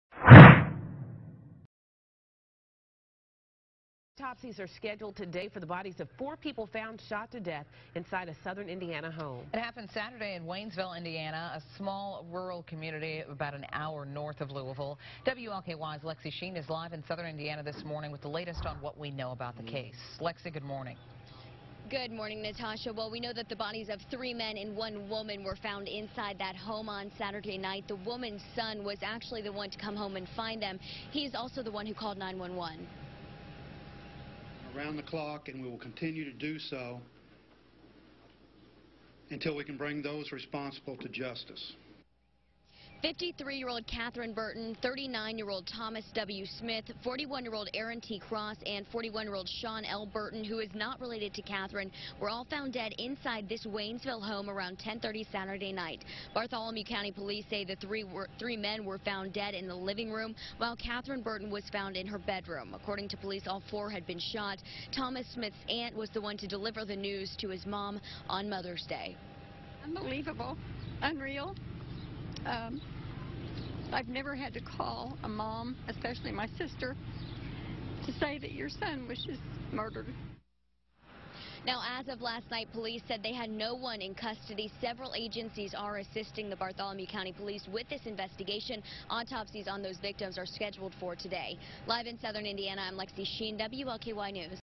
Autopsies scheduled for 4 bodies found in Waynesville home
Autopsies are scheduled for the bodies of four people found shot-to-death inside a southern Indiana home.